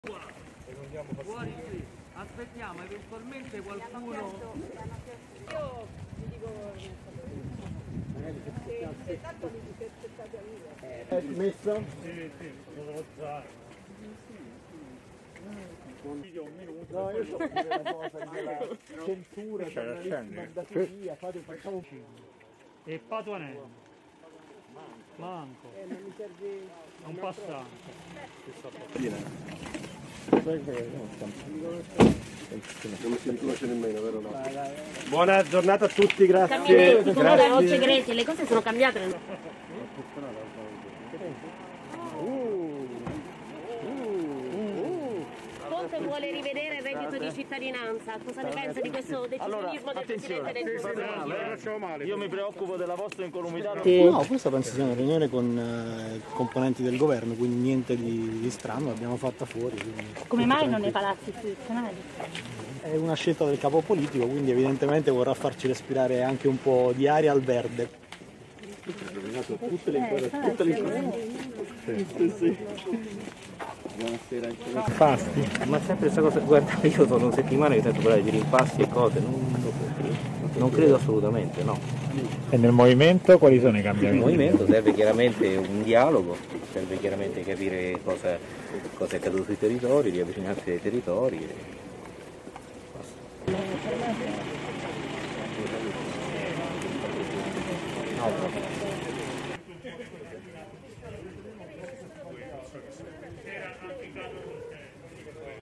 M5s, al via la riunione "segreta" fuori Roma
Presenti ministri e sottosegretari grillini. Si apre la strada agli Stati Generali

POLITICA (Roma). "Questa penso sia una riunione con i componenti del Governo. Farla qui è una scelta del capo politico, probabilmente vuole farci respirare un po' di aria", così Carlo Sibilia, viceministro all'Interno, in un agriturismo fuori Roma dove si sta svolgendo una riunione della squadra di Governo M5s per fare il punto sulla riorganizzazione del Movimento e sull'azione dell'esecutivo. "Sono settimane che sento parlare di rimpasti. Non credo assolutamente. Nel Movimento serve un dialogo e capire cos'è accaduto nei territori", ha detto Pierpaolo Sileri, viceministro alla Salute. (l.sassi)